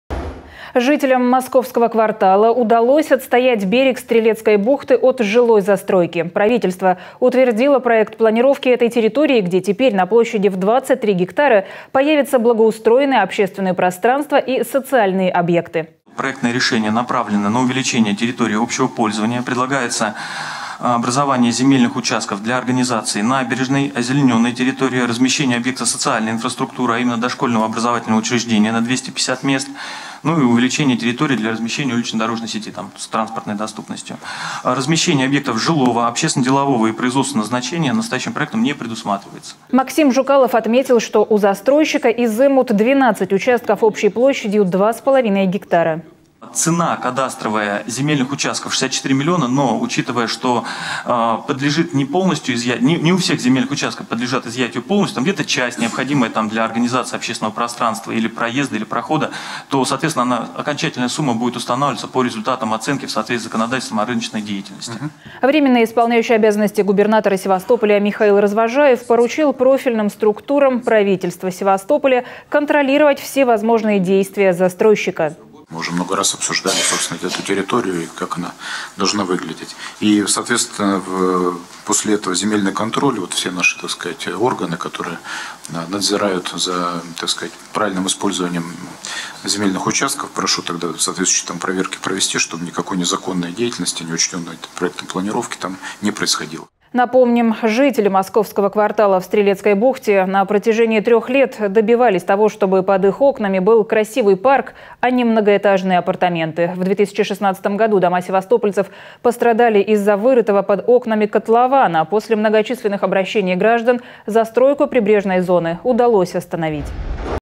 Жители «Московского квартала» защитили берег Стрелецкой бухты от застройки
Жителям «Московского квартала» удалось отстоять берег Стрелецкой бухты от жилой застройки. Правительство утвердило проект планировки этой территории, где теперь на площади в 23 гектара появятся благоустроенные общественные пространства и социальные объекты.